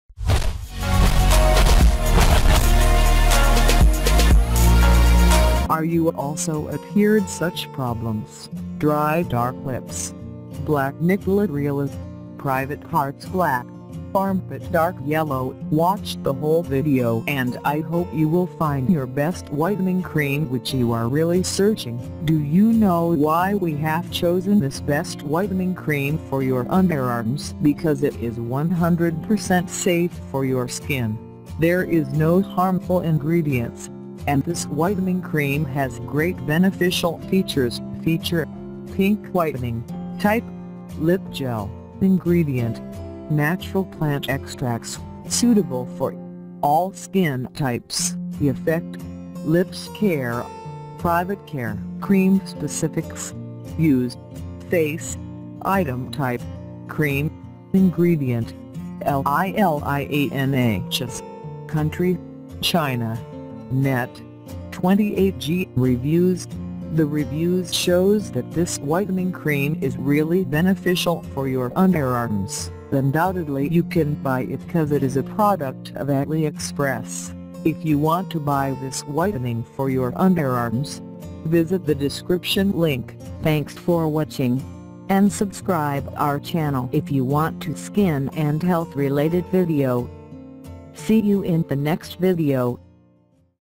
Whitening Cream For Underarms | Best Whitening Cream For Underarms - Healthcare Review
About the video:
In this video I  reviewed whitening cream for underarms. This is for female. Female can use this whitening cream in their underarms and private areas. If you want to buy this whitening cream for your underarms, visit the description link on the above.